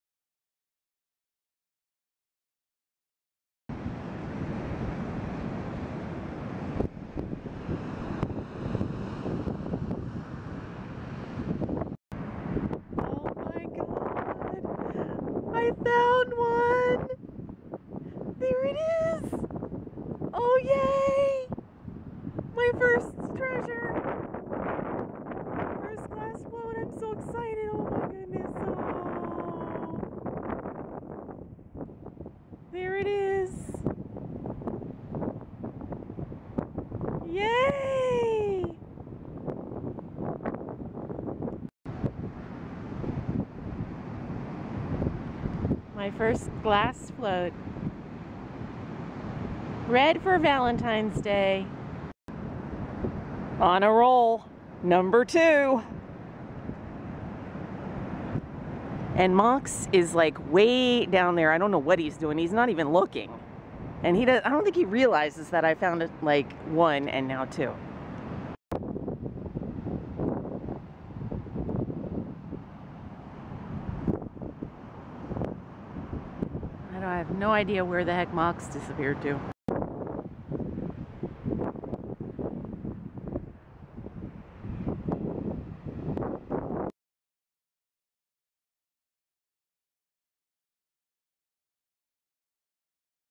I Spy...Treasure!!
The coveted special red glass floats - just in time for Valentine's Day!